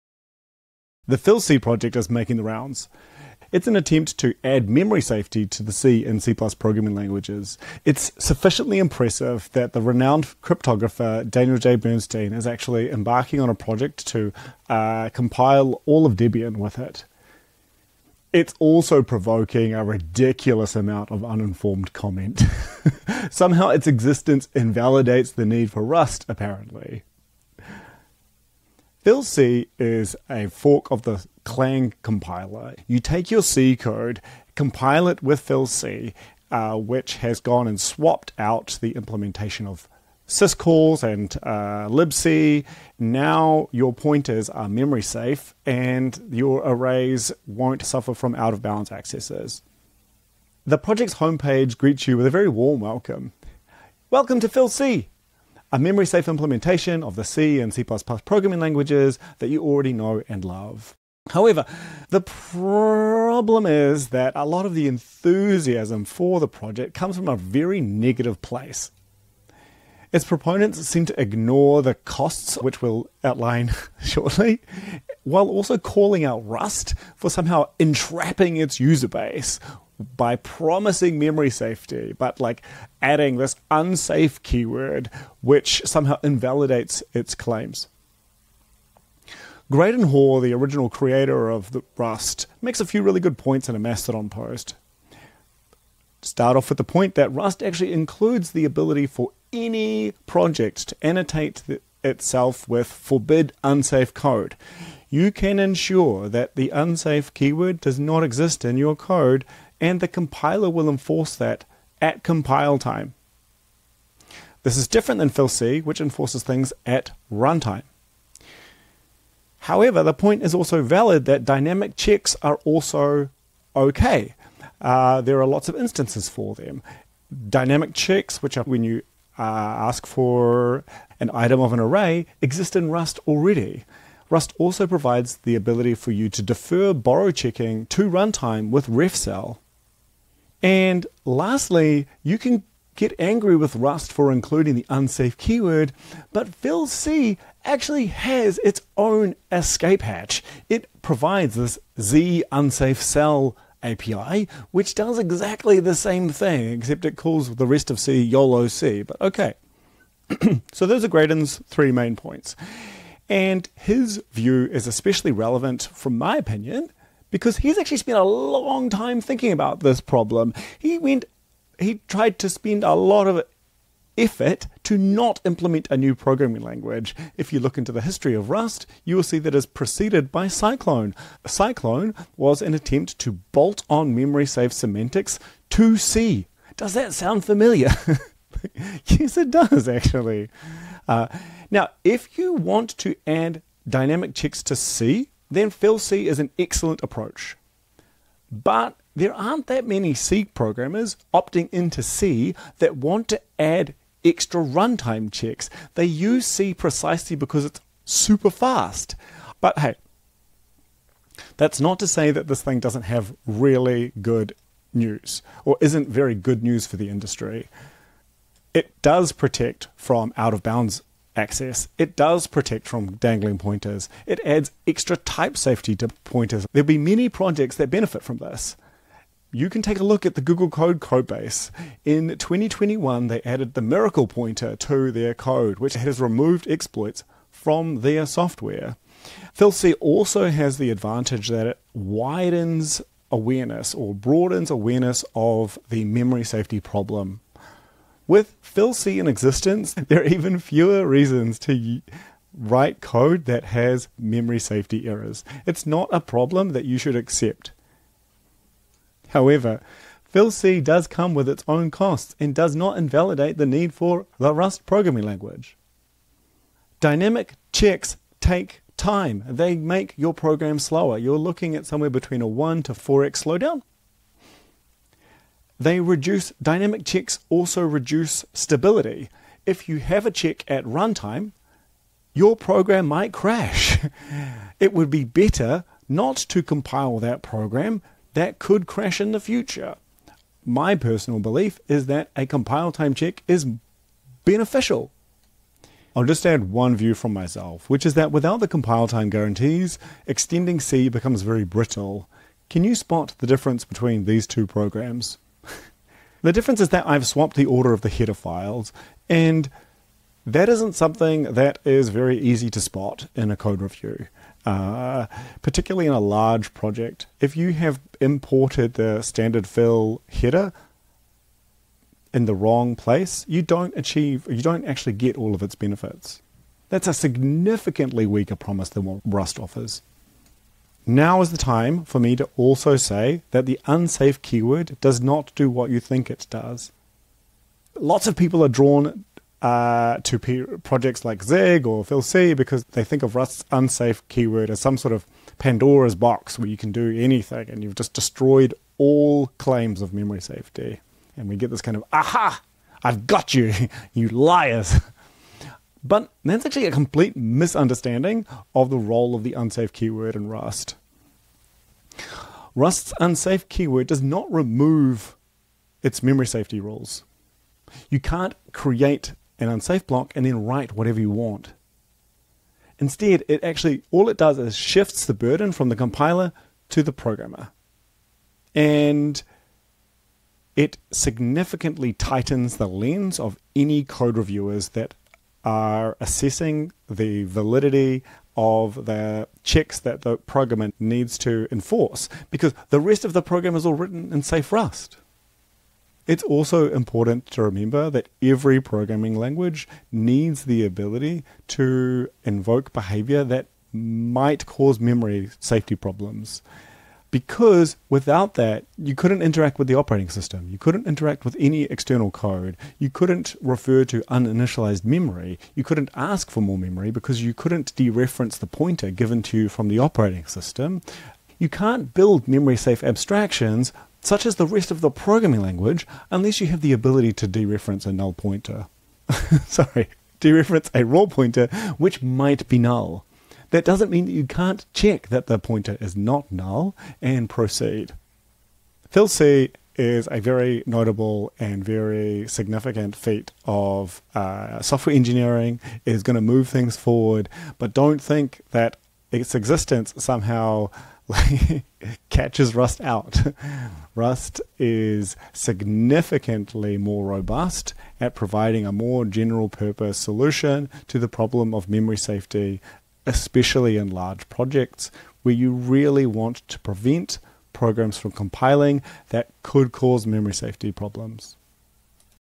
Fil-C is a better Rust
Is Fil-C the solution to C/C++ memory safety and does it replace the need for Rust?

Fil-C, a fork of the Clang compiler, is gaining traction for its ability to add memory safety to existing C code via dynamic runtime checks. While it’s a massive engineering feat, it has sparked a debate about the necessity of Rust.

This is all generating significant buzz. And a lot of uninformed hot takes (including "Rust is less safe than Fil-C in practice" from its creator Filip Pizlo). This video breaks down what Fil-C actually does, its genuine benefits, and why it doesn't invalidate Rust's approach to memory safety.

Key Points Covered:

- How Fil-C achieves memory safety through dynamic checks
- The actual costs: 1-4x performance overhead and reduced stability
- Why compile-time guarantees matter, especially in large projects
- What Rust's unsafe keyword actually does (hint: not what critics think)
- Why every language needs an "escape hatch" for OS interactions

Fil-C is excellent news for the C/C++ ecosystem and broadens awareness of memory safety issues. Projects like Chrome's MiraclePtr demonstrate real-world value. However, runtime checks and compile-time guarantees the problem differently. Large production code bases benefit enormously from catching issues before deployment.

Timeline

00:00 Fil-C the "Rust Killer"
00:28 What is Fil-C?
01:01 Rust already provides what Fil-C offers, and mostly at compile time
02:47 Not C's first rodeo: Cyclone, MiraclePtr
04:19 Significant downsides of dynamic checks
05:30 "Unsafe" in Rust is more safe than you think